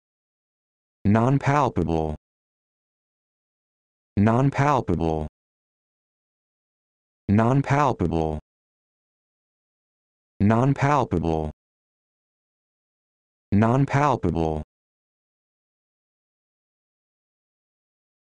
How to Pronounce Nonpalpable
This video shows you how to pronounce Nonpalpable